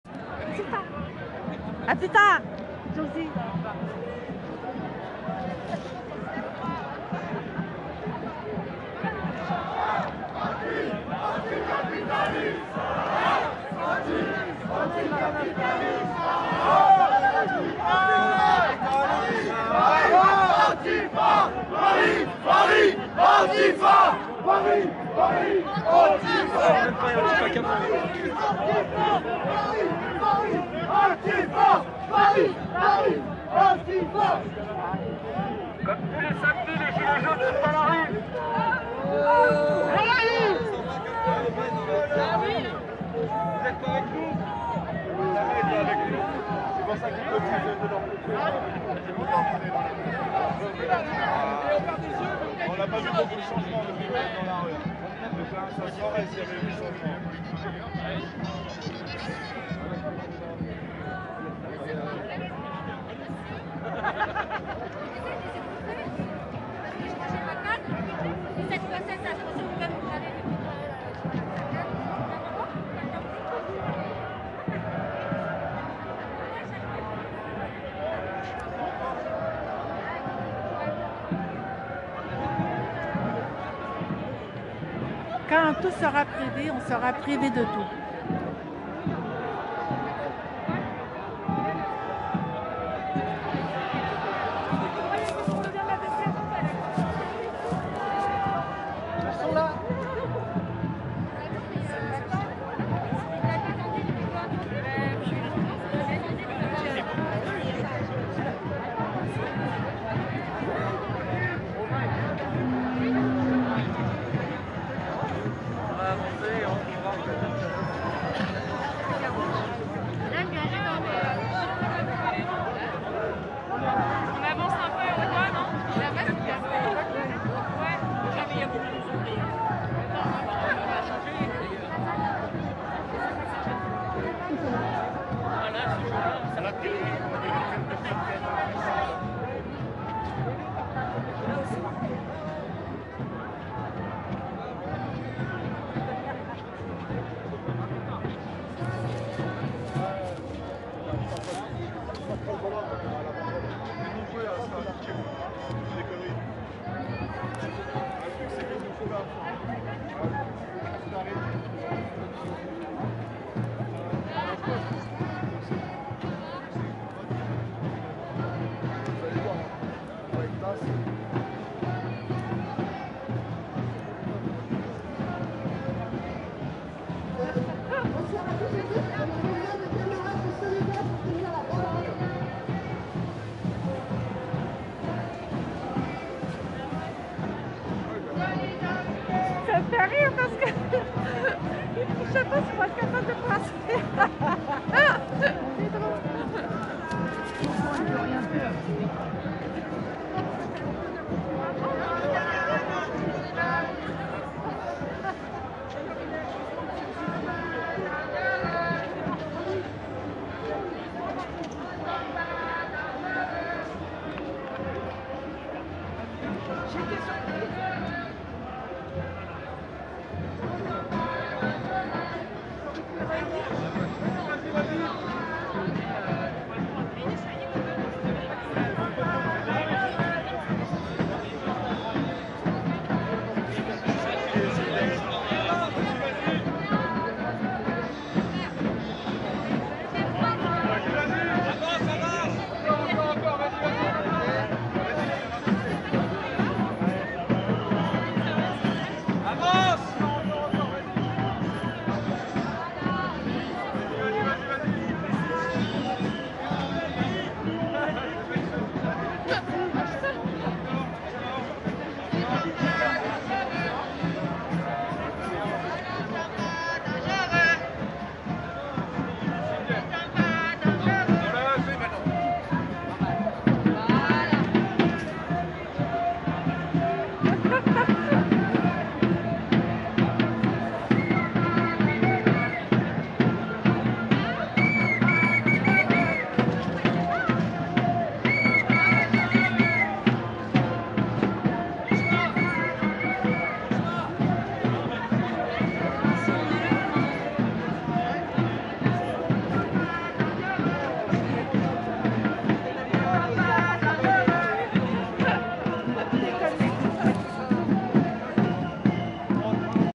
Manif Paris Republique 1ermai 2021 mariepanic no.1
Manif Paris Republique 1ermai 2021 mariepanic no.1 onlacherien giletjaune onarretetoutes Josie cgt 1ermai2021 samedi 1er Mai 2021
+ Samedi 1er Mai 2021 ) 1ermai2021 Manifestation au départ de Place des Fêtes (Paris 19e) 12h + Manifestation au départ de Place de la République (Paris 10e) 14h + La ligne de la Legal Team Paris sera ouverte pour le 1er mai 2021 Pour les urgences, si un·e de tes proches se fait interpeller, tu peux appeler le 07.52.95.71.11